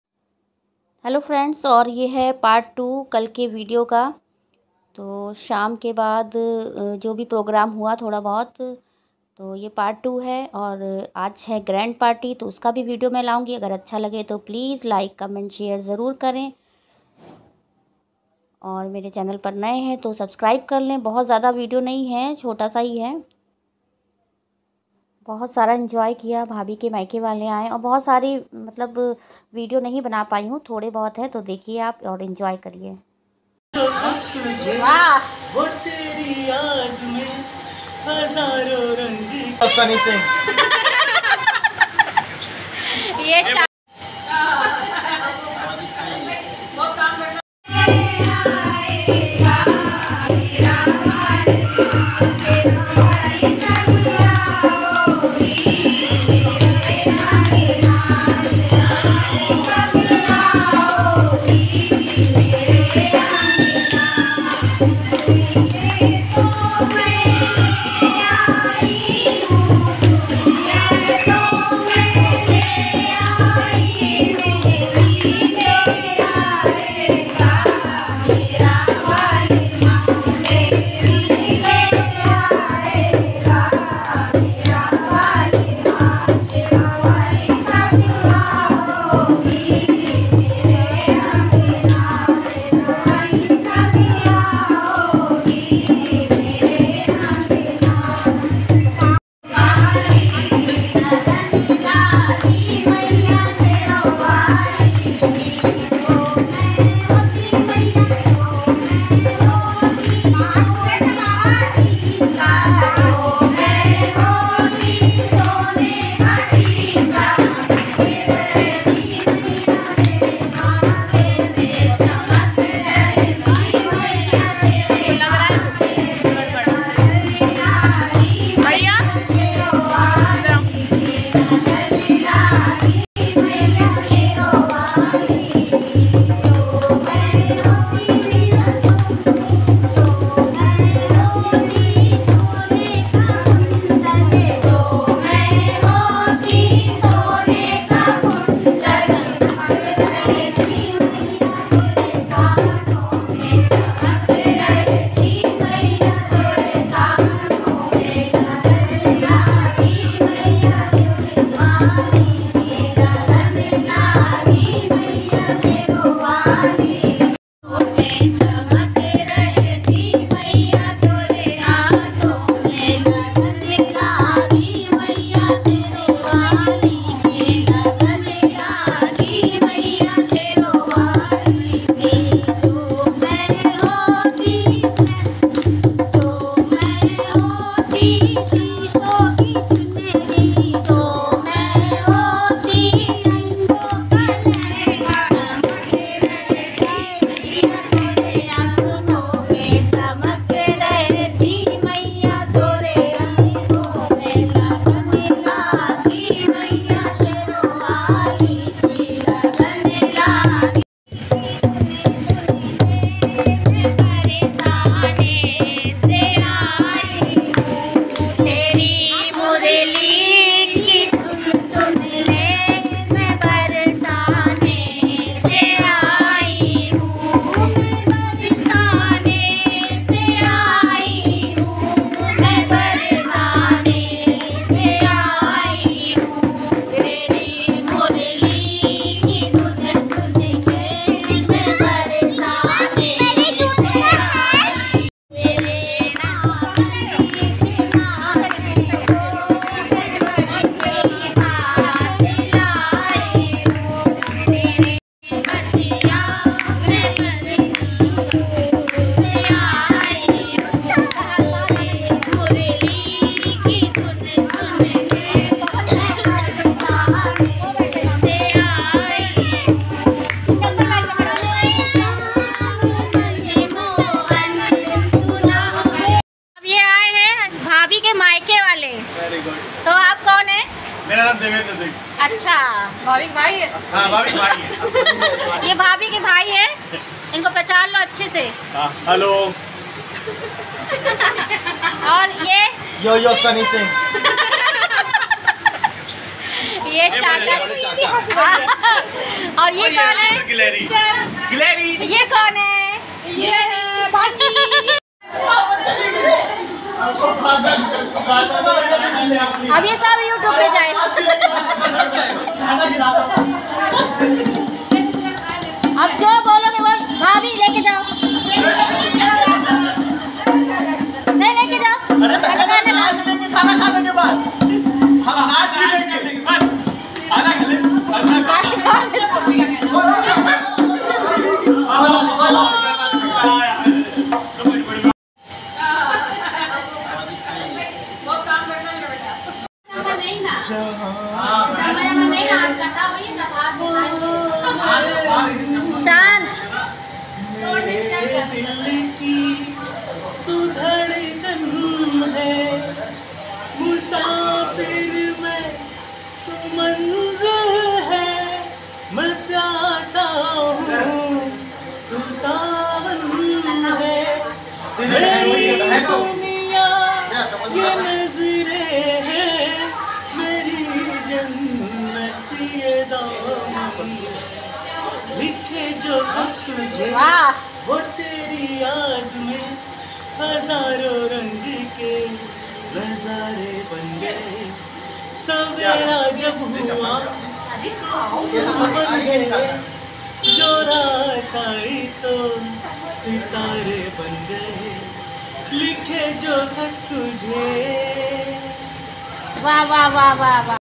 anniversary vlog pary -2 mayke walo n Gaya gana ❤️🥰 💐🎁happy family ❤️🌹shortvideo
ye anniversary ka part 2 h party ka video next vlog m ayega please is video ko pyar de 🙏🙏🥰❤️🌹💐🎁